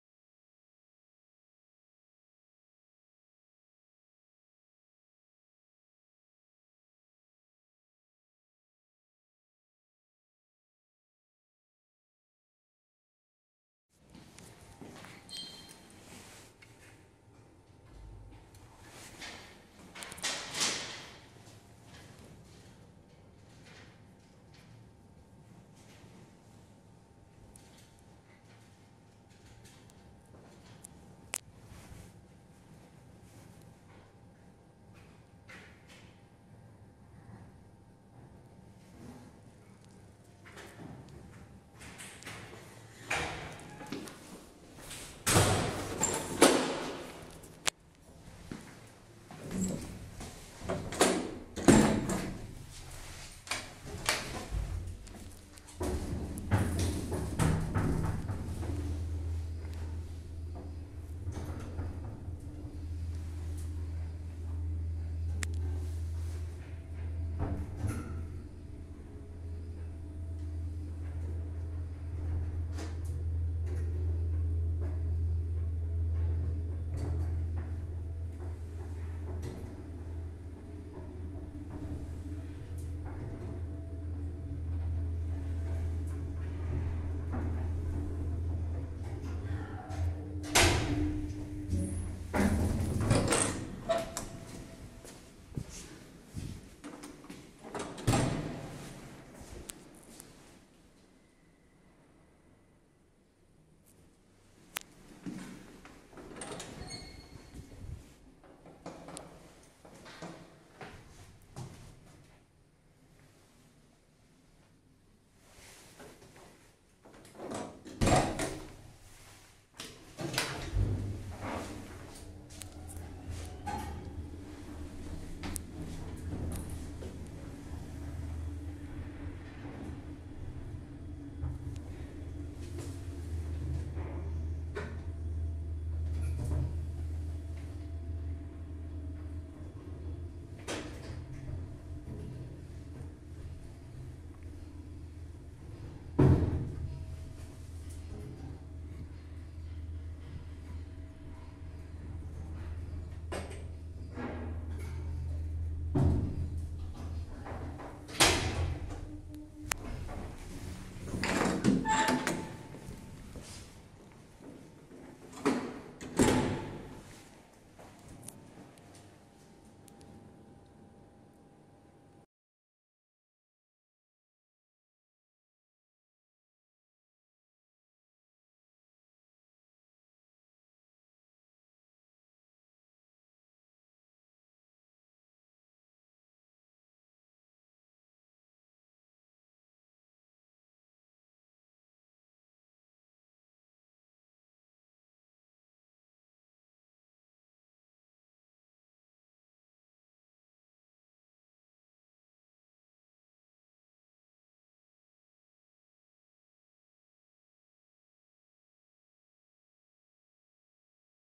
Original 1966 Flohr-OTIS freight elevator at Baumwollbörse in Bremen, Germany
This is an old office building from the 1920s. Well, the elevator is not as old as the building is, but still very classic. It still doesn't have got inner doors, which makes it even more classic.